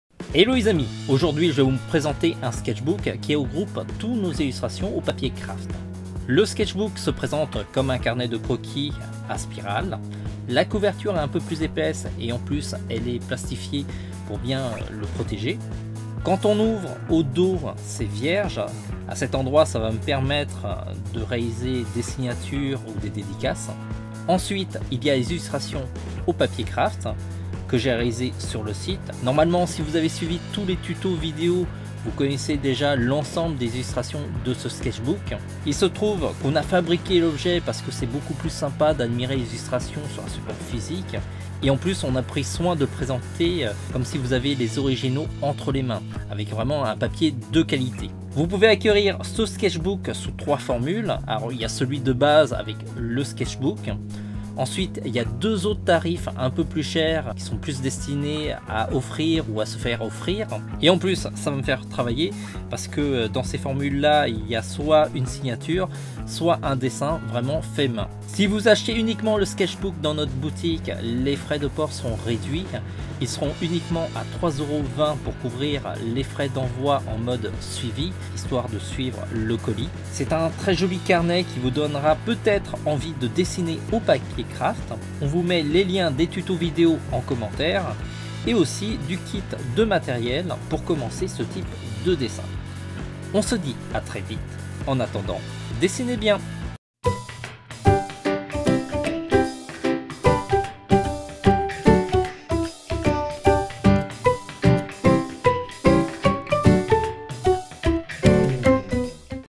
Sketchbook TVHLAND des dessins au papier Kratf
Voici une présentation de notre sketchbook au papier kraft.